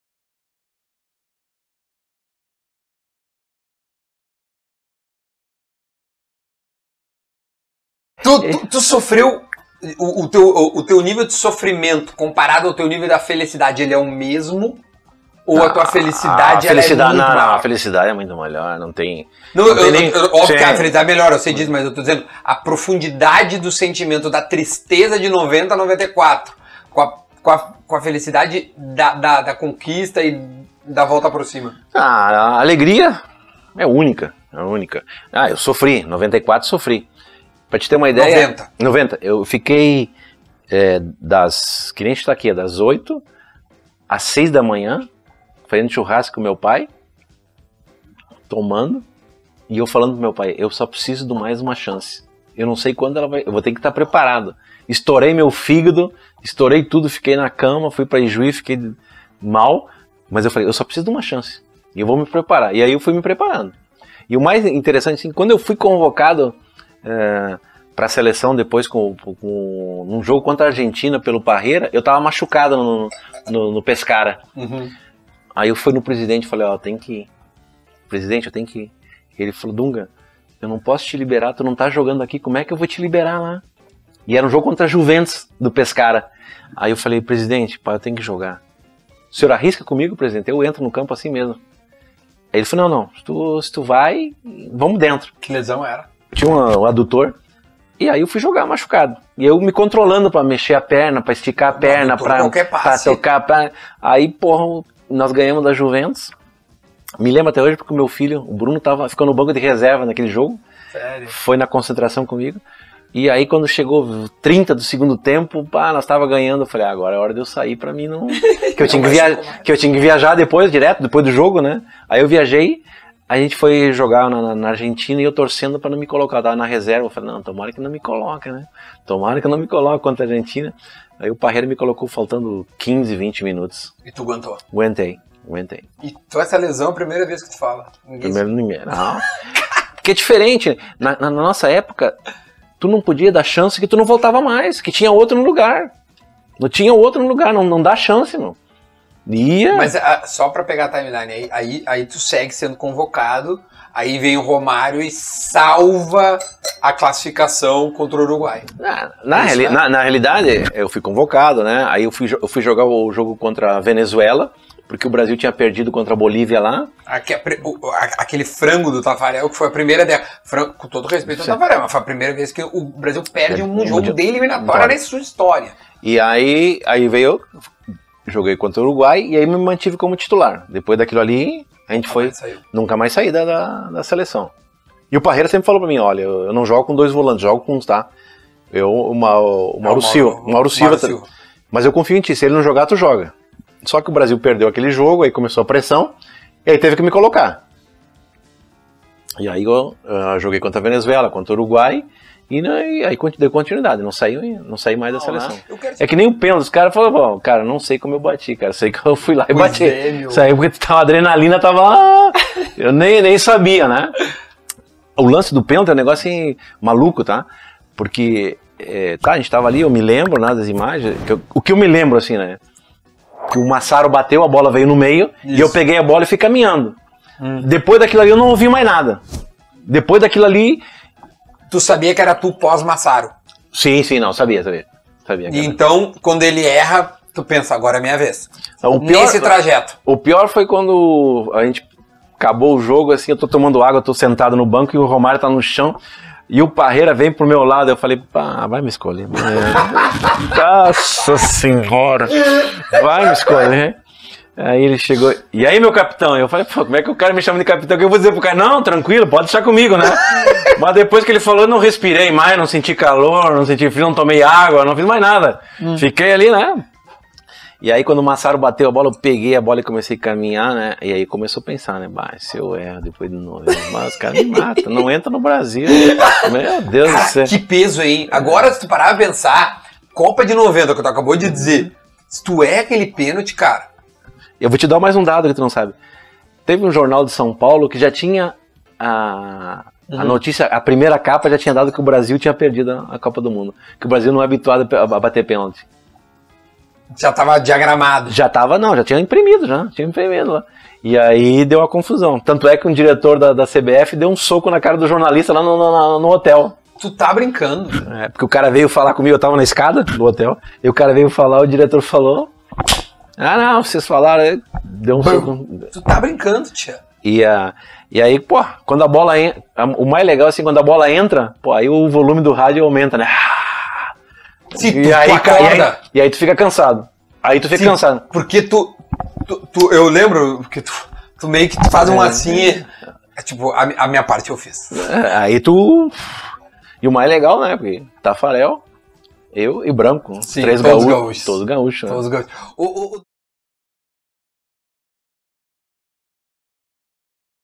Dunga conta histórias da Copa de 94
“Você não pode colocar um limite em nada. Quanto mais você sonha, mais longe você chega.”